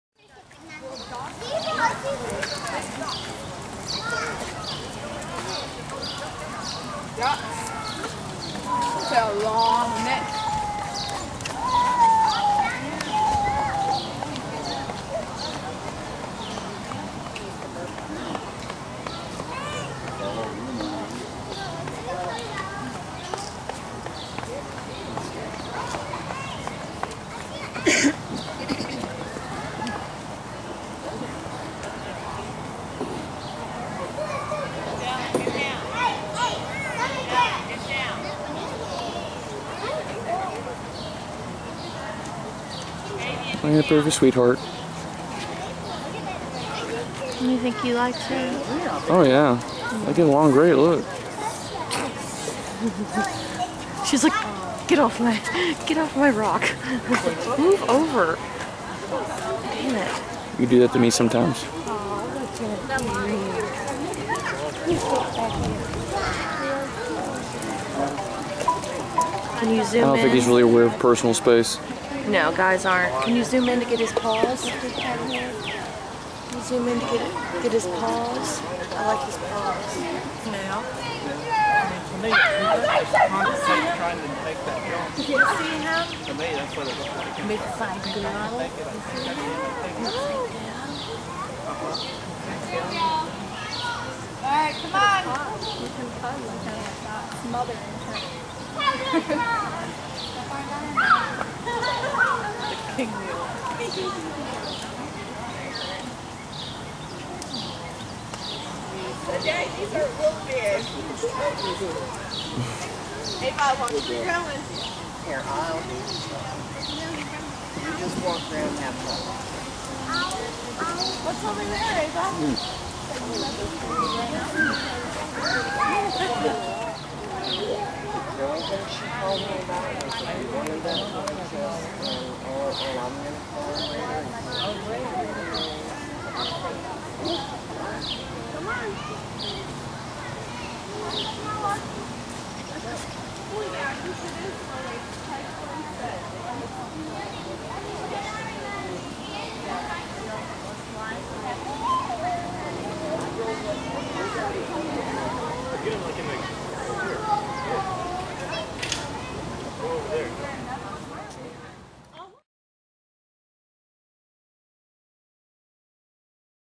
A Bored Lion and Lioness Cuddle
A bored lion and lioness cuddle at the zoo. Filmed by Jeremy Patton with Holly Koala Jett, June 15, 2013.